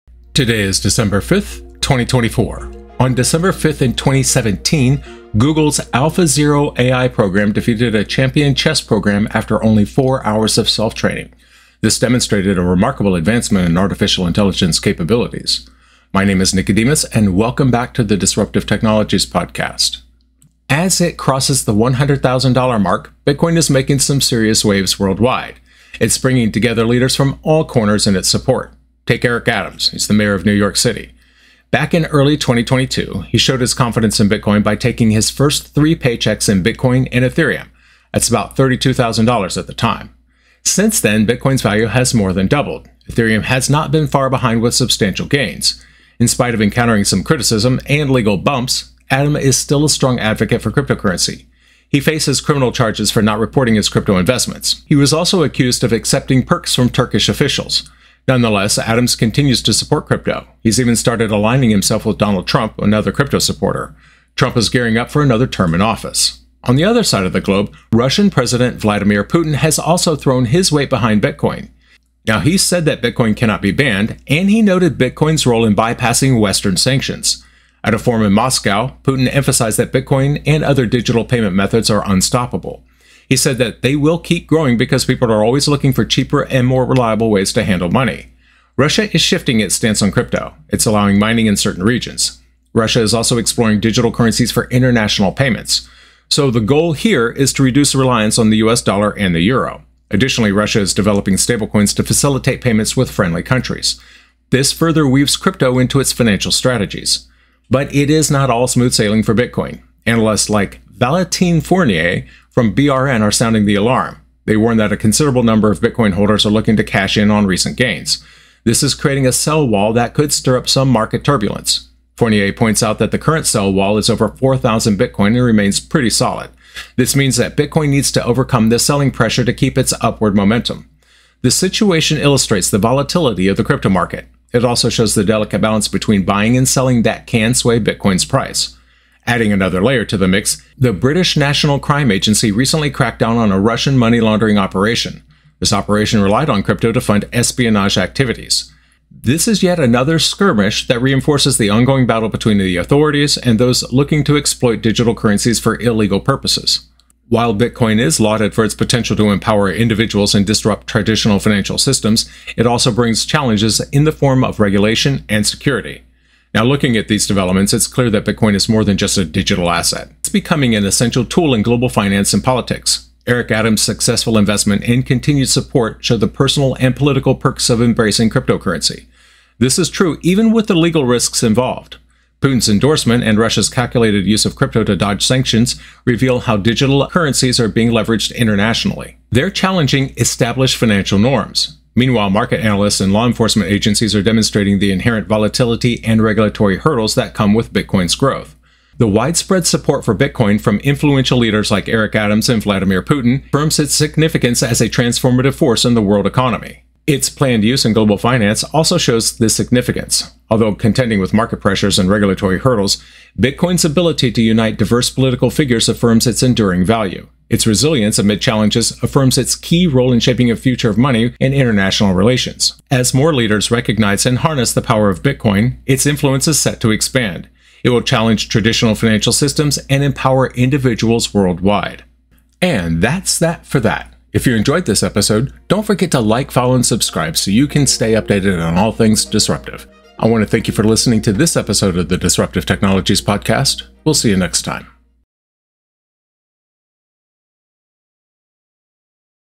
Eric Adams and Putin Back Bitcoin
From New York to Moscow, Bitcoin is winning over leaders worldwide. Explore how Eric Adams, Vladimir Putin, and others are leveraging crypto for political and financial strategies.